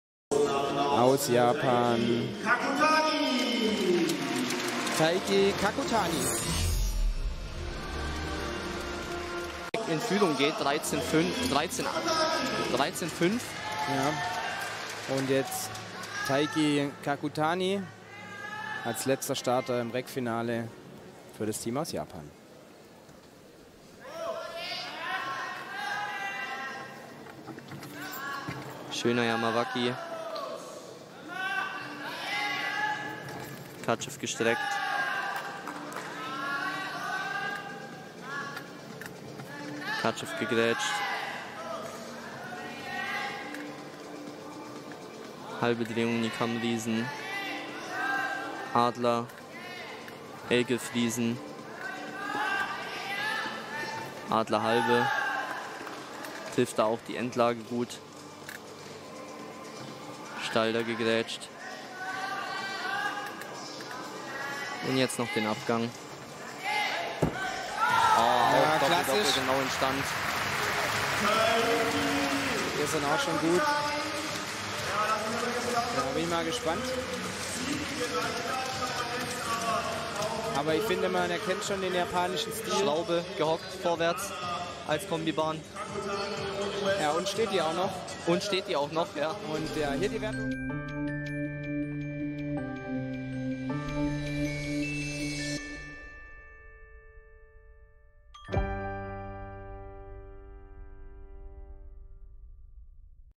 39th EnBW DTB-Pokal. Junior Apparatus Final. 🥈 HB KAKUTANI Taiki (JPN)
39. EnBW DTB Pokal | Junior Apparatus Final

Horizontal Bar Final

🥈 KAKUTANI Taiki
HB D 4.6 E 8.833 Score 13.433

Stuttgarter Porsche-Arena
Stuttgart (GER). 16/03/2024